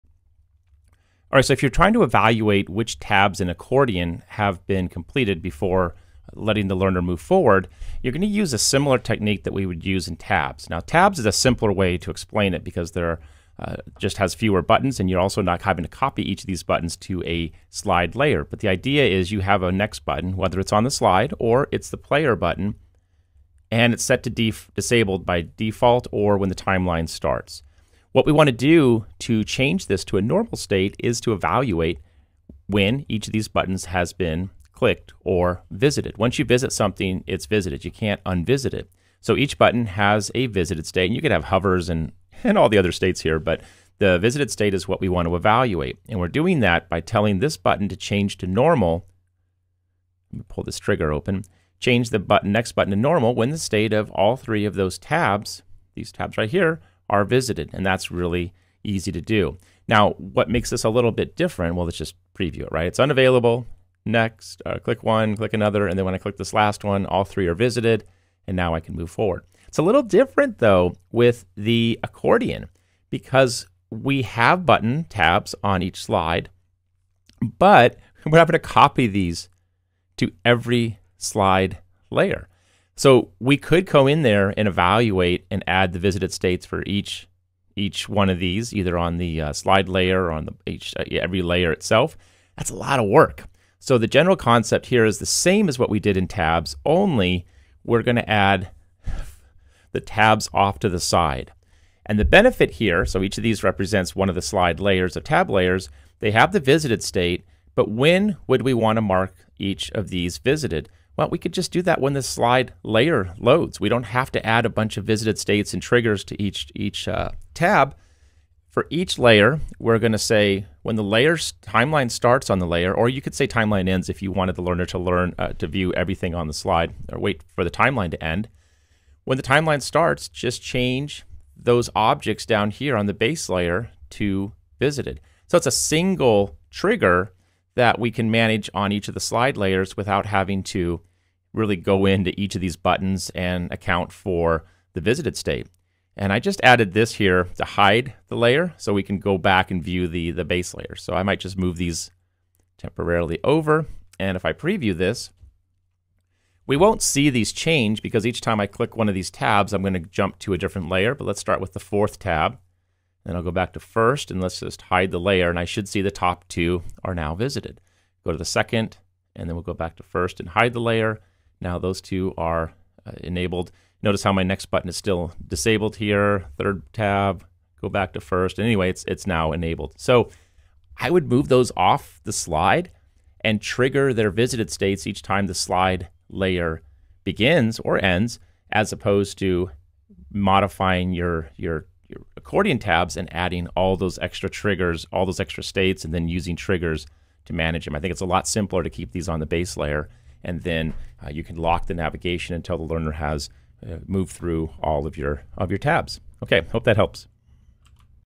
Accordion Interactions with Conditional Next Button in Storyline 360
When developing e-learning courses in Articulate Storyline 360, it's important to consider how you can keep learners engaged with the content. One way to do this is to disable the "next" button until learners have clicked all of the accordion button tabs on the slide. This ensures that learners are interacting with the content and prevents them from moving on too quickly.

Here's how you can disable the next button in Storyline 360 until learners click all button tabs in an accordion interaction built with Articulate Storyline 360.